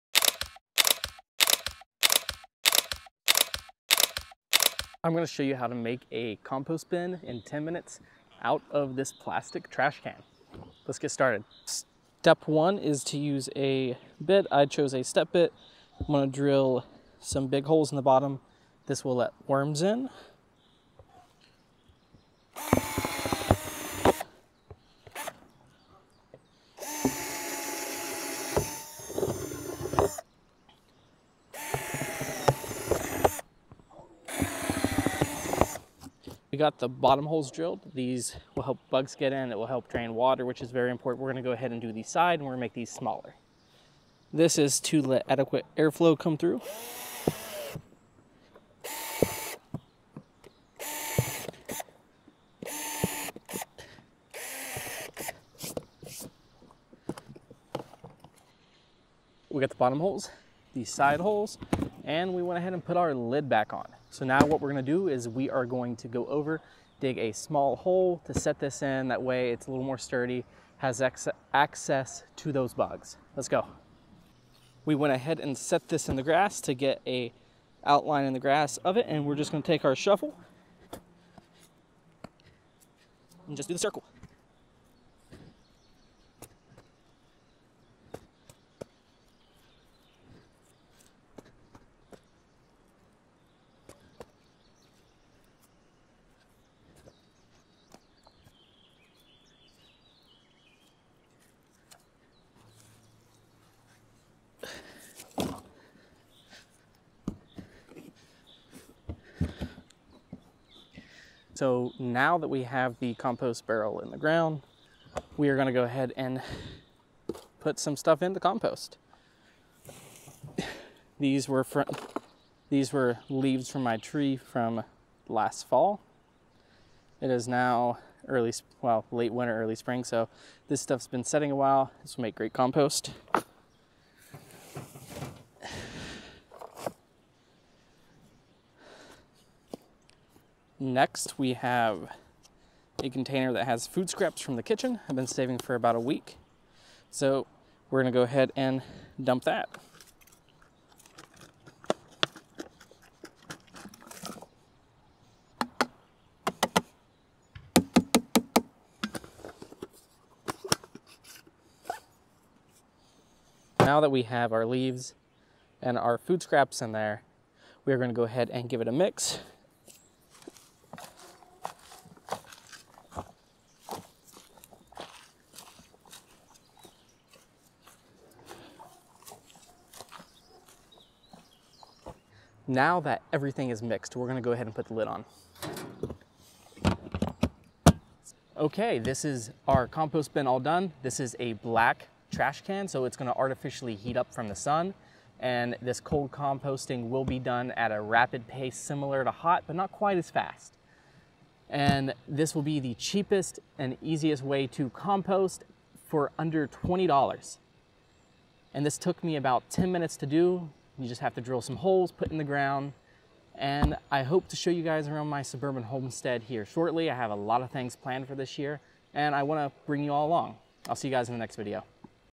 How To Make a Compost Bin Trash Can Fast
Make a Compost Bin fast and transform your garden. This video will guide you through the process of converting an everyday household item into a sustainable solution for waste management. Learn how to turn your kitchen scraps into nutrient-rich compost. Perfect for beginners and seasoned gardeners alike.

32 gal Trashcan

Step bit

pre-made square compost bin

rotating compost bin

cheapest big compost bin